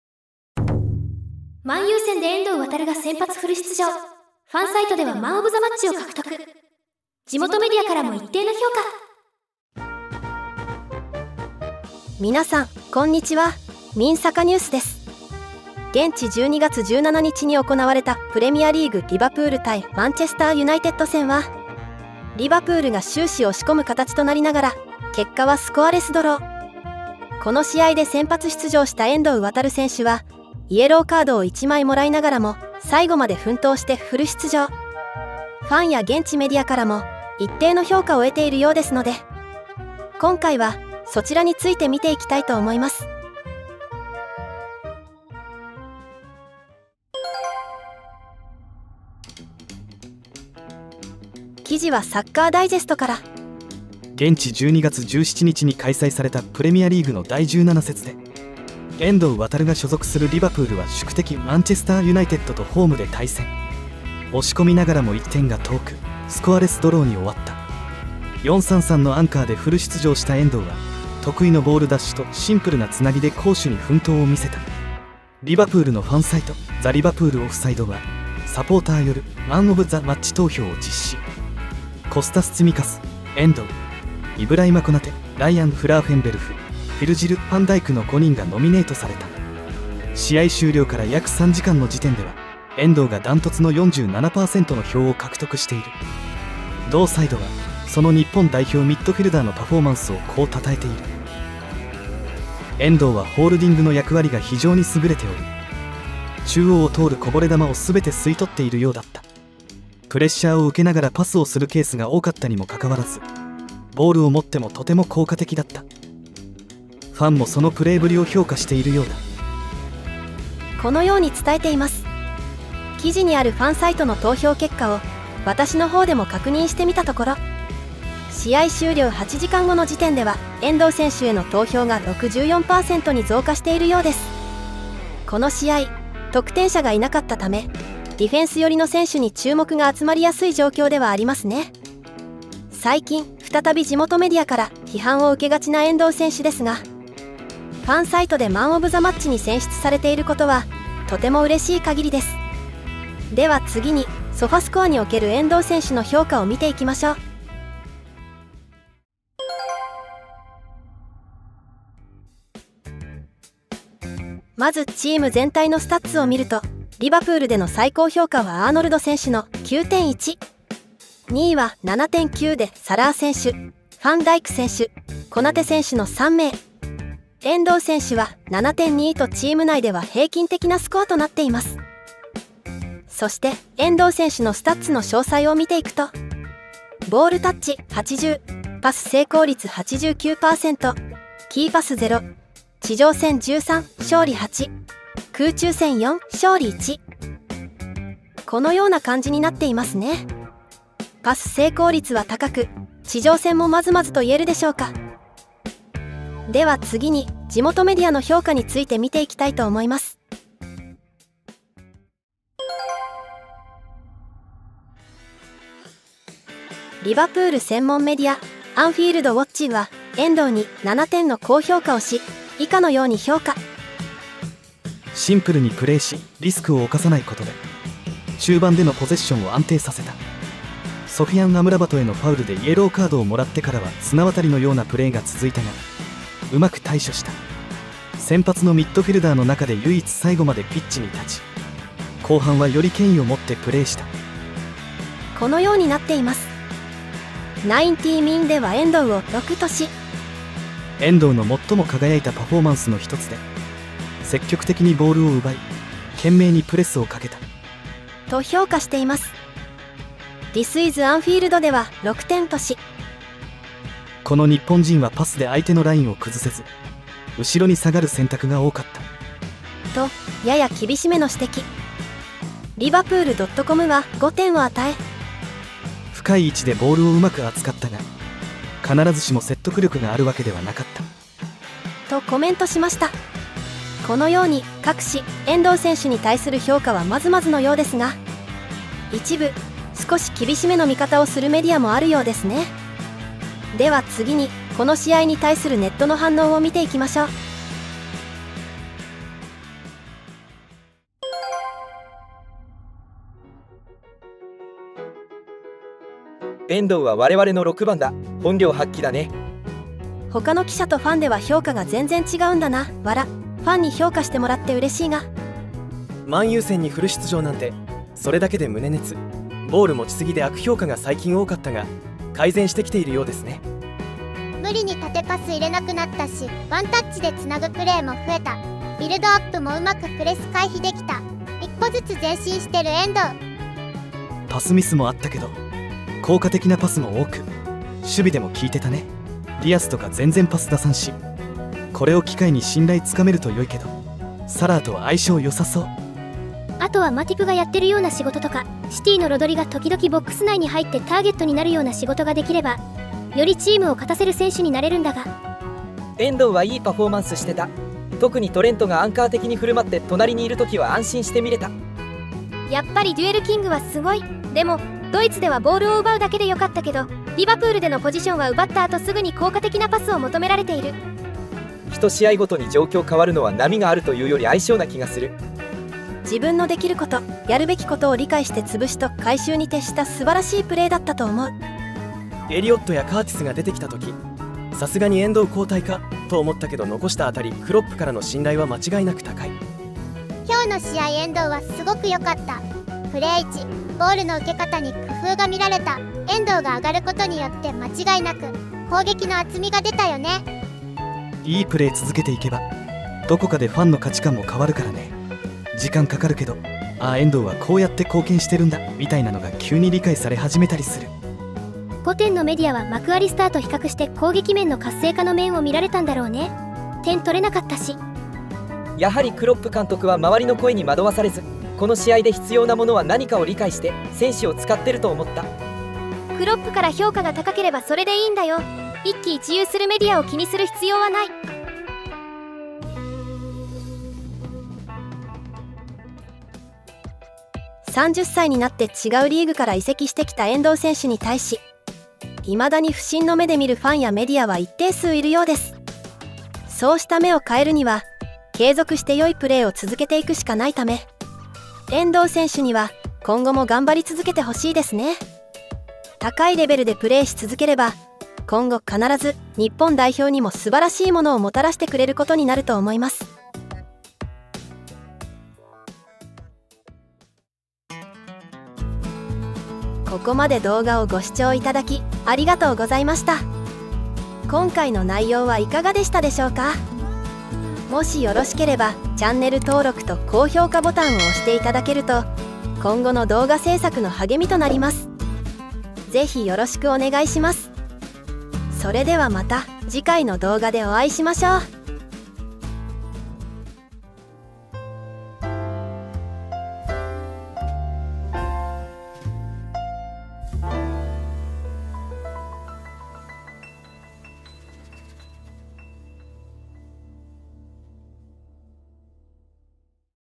【ダントツ1位】遠藤航がマンU戦のダービーで躍動! 地元ファンによるMOMに1位で選出!!
いつも、ご視聴いただきありがとうございます！

12月17日に行われたプレミアリーグ  リバプール vs マンU戦で躍動し、地元のファン選出のMOMに選ばれました。

今回は、そちらに対する内容とネットの反応をまとめましたので

引用元：
サッカーダイジェスト